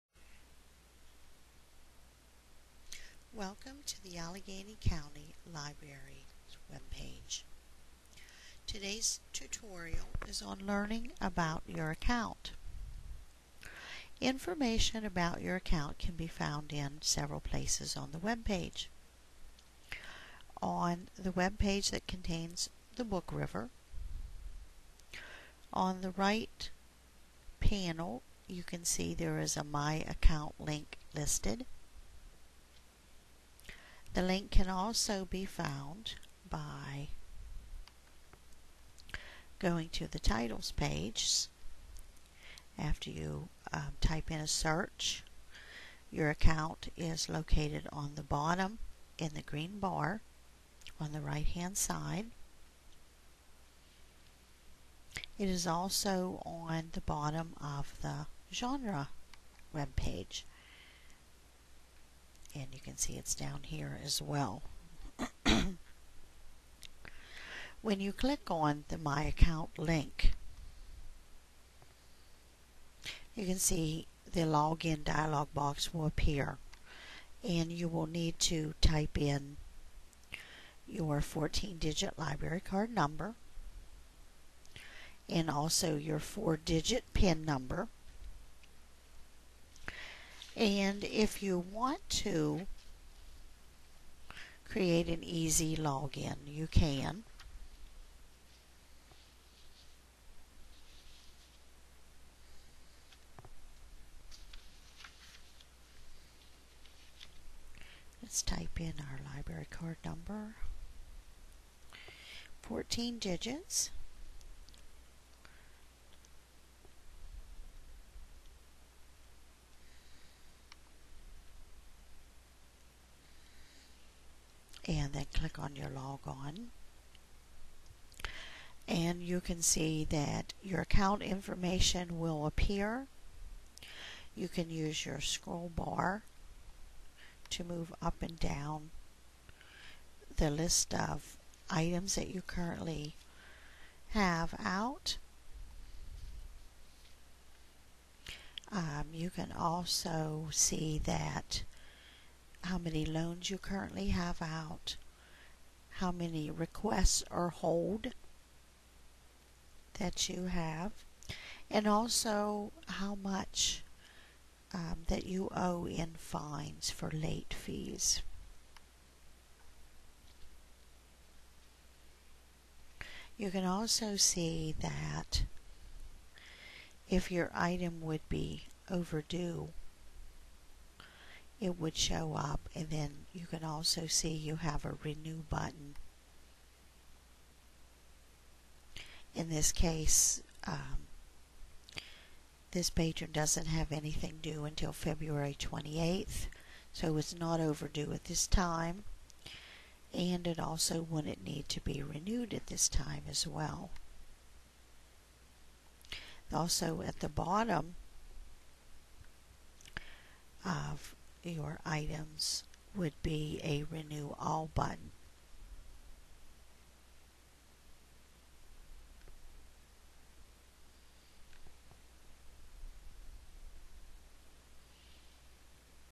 Allegany County Library System - My Account Information Tutorial
Learn where and how to find your personal account information on our library website.